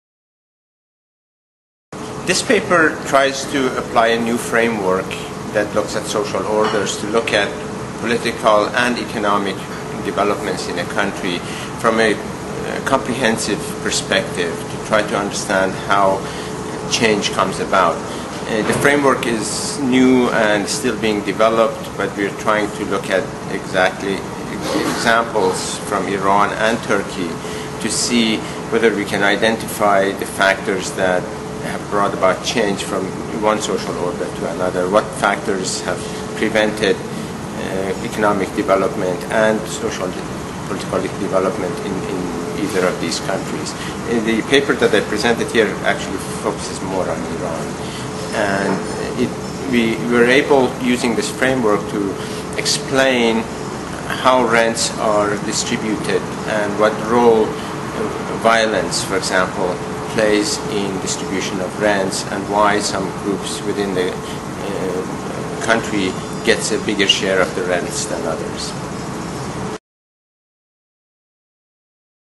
What drove changes in political settlement in Iran and Turkey?
In this video, Hadi Esfahani (University of Illinois, USA) presents a new framework that looks at social orders in order to understand how political and economic changes are brought about in a country.

ERF workshop "The political economy of transformation in the ERF region"
27-28 October, 2013
Tunis, Tunisia